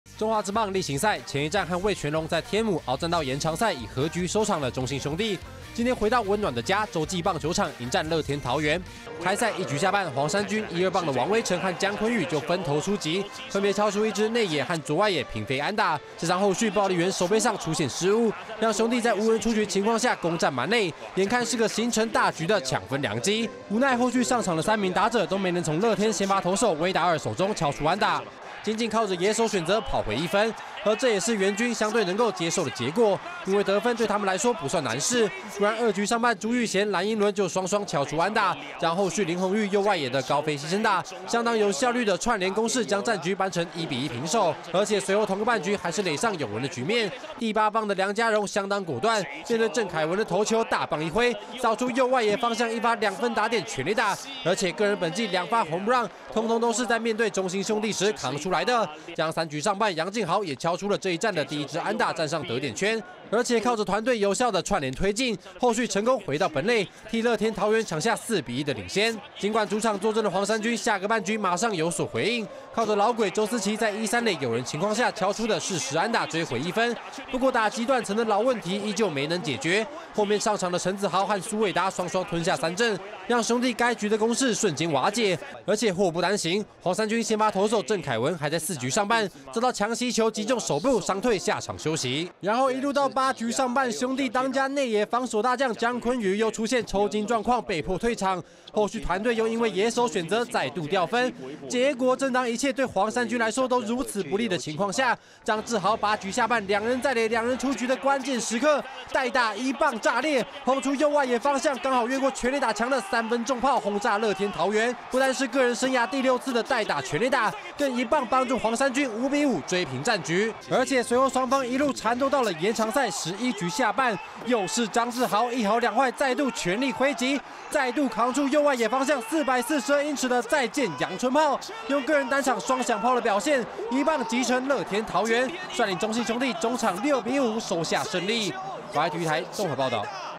6/9 中信主場交手樂天蜘蛛豪炸裂雙響砲 延長再見轟助兄弟6:5擊沉樂天
中信兄弟在今天交手樂天桃猿的比賽中最關鍵的人物，絕對就是在八局下半，一棒扛出追平三分砲的張志豪。

沒錯，而且蜘蛛豪是真的狂，隨後延長賽十一局下半，又是張志豪扛出一發再見全壘打，個人繳出單場雙響砲的表現，率領兄弟6:5收下勝利。